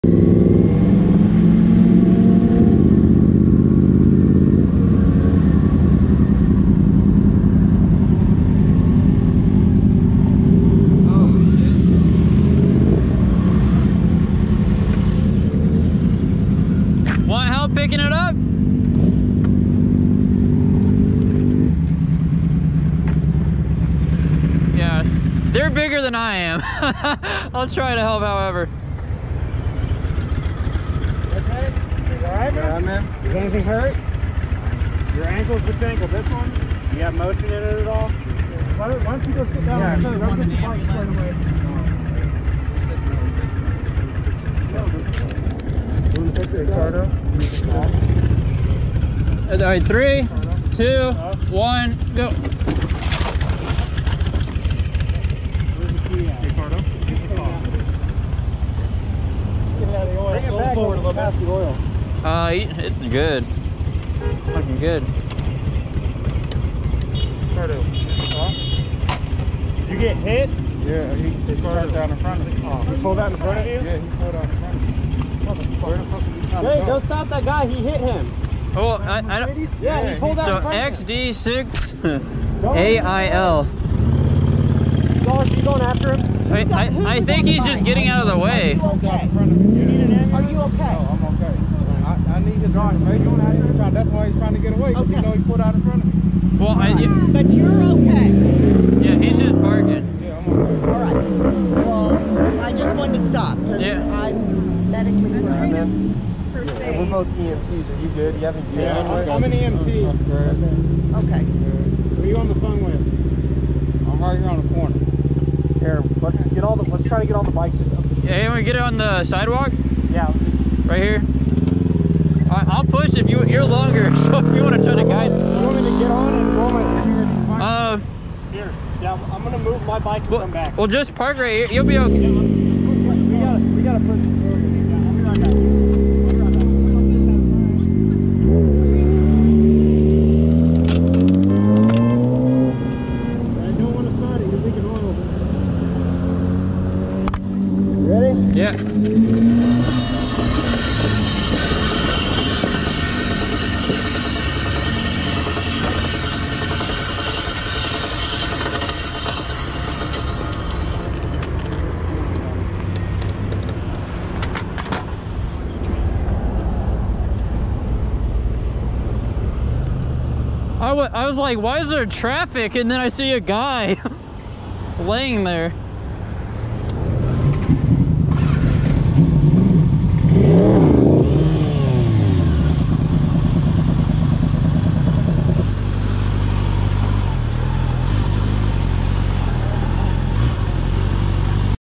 Motorcycle Accident Middle Of Westport Intersection | Quick Clips #1
Out on a chill Sunday Cruise and came up on a guy sitting on his motorcycle. Said he had the green light, car came through and hit him. He is okay, bike damage can be seen in video.